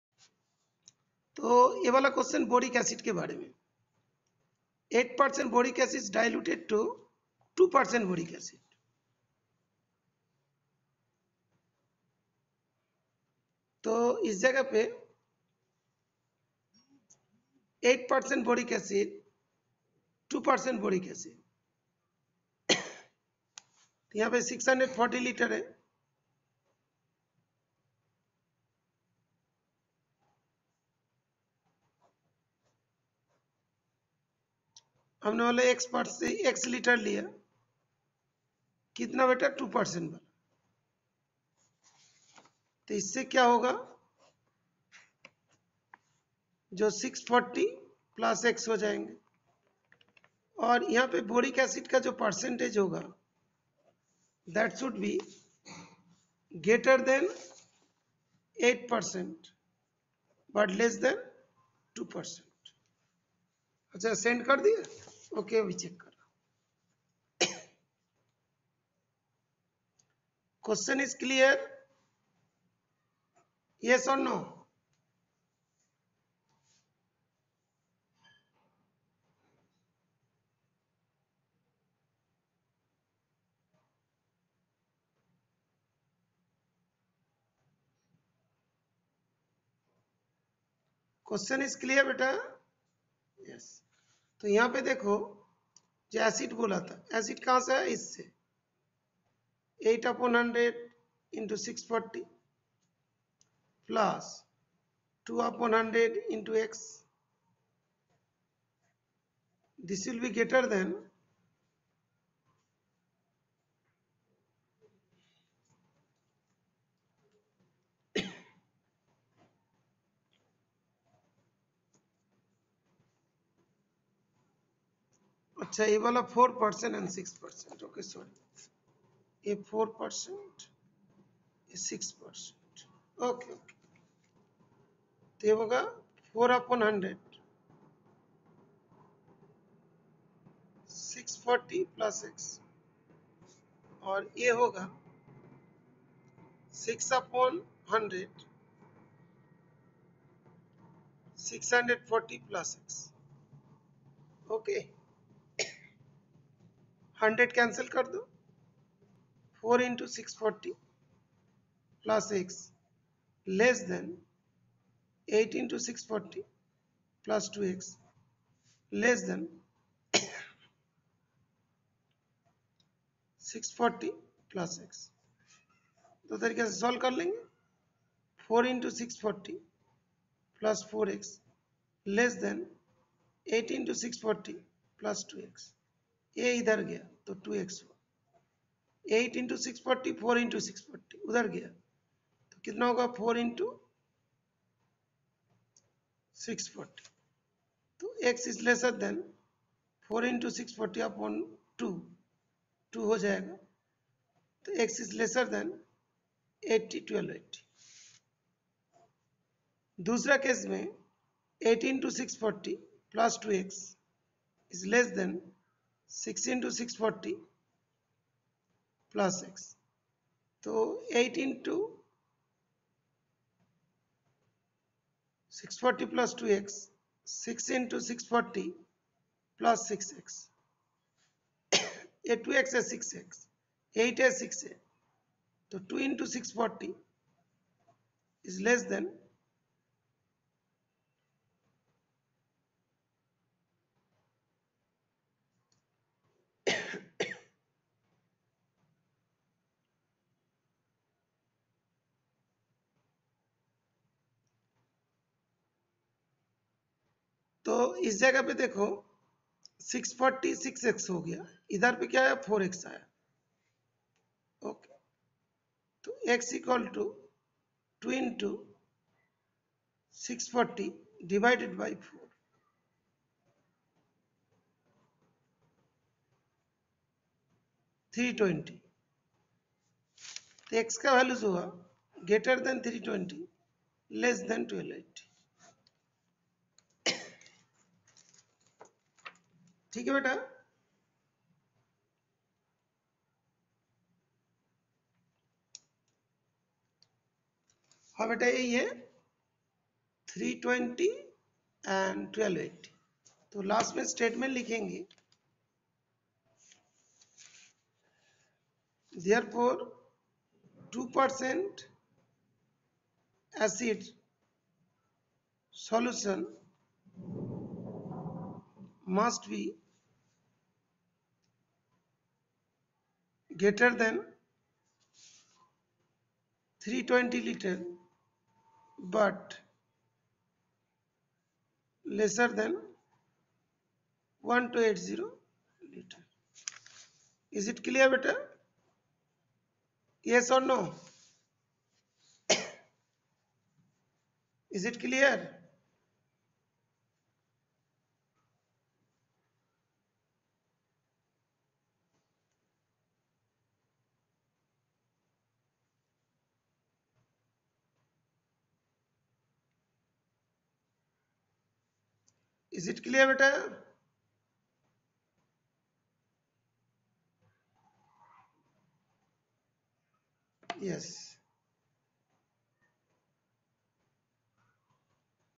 11maths linear inequalities
12. A solution of 8% boric acid is to be diluted by adding a 2% boric acid solution to it. The resulting mixture is to be more than 4% but less than 6% boric acid. If we have 640 litres of the 8% solution, how many litres of the 2% solution will have to be added?